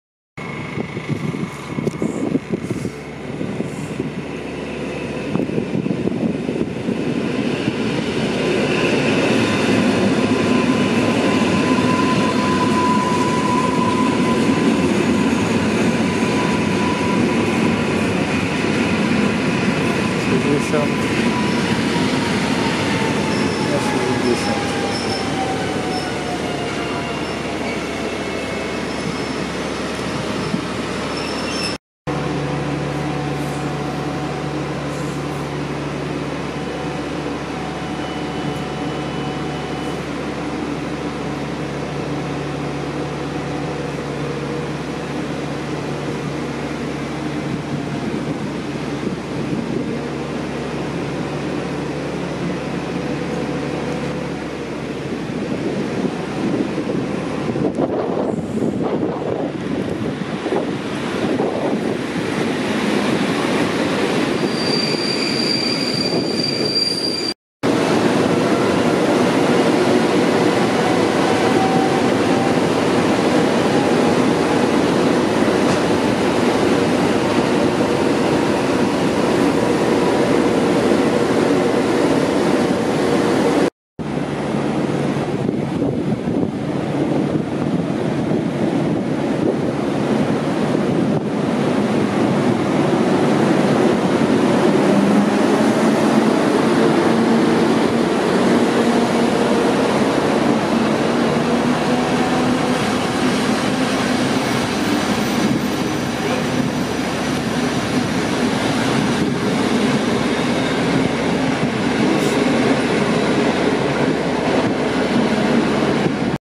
EN57AKM i TLK Kochanowski. Łowicz Gł. 13.03.2020.
EN57AKM-1660+1590 kończy bieg.
EP07-411 z TLK Kochanowski do Chełma.
Trwają intensywne prace przy budowie peronu 2. W rejonie torów postojowych pracuje palownica. Jest wzmacniany grunt.